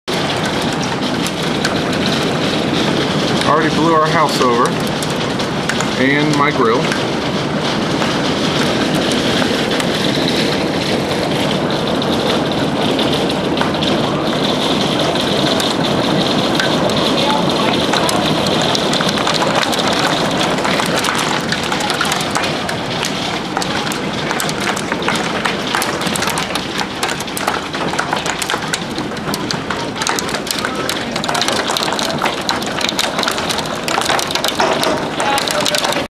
Hail and Wind 2 of 3
Unusual amount of rain mixed with hail and strong winds suddenly springs up in Nampa, ID.